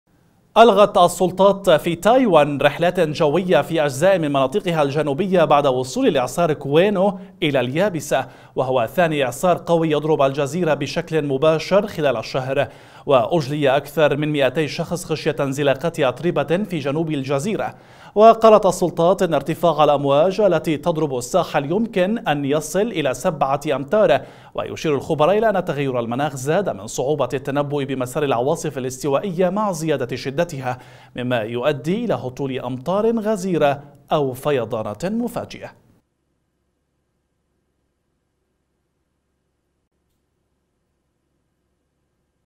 إعصار "كوينو" تايوان برياح "قياسية"
ألغت السلطات في تايوان رحلات جوية في أجزاء من مناطقها الجنوبية بعد وصول الإعصار كوينو المتوقع إلى اليابسة ، وهو ثاني إعصار قوي يضرب الجزيرة بشكل مباشر خلال  شهر.  وأ جلي أكثر من مئتي شخص خشية انزلاقات أتربة في جنوب الجزيرة ، وقالت السلطات إن ارتفاع الأمواج التي تضرب الساحل يمكن أن يصل إلى سبعة أمتار . ويشير الخبراء إلى أن تغير المناخ زاد من صعوبة التنبؤ بمسار العواصف الاستوائية، مع زيادة شدتها، مما يؤدي إلى هطول أمطار غزيرة أو فيضانات مفاجئة.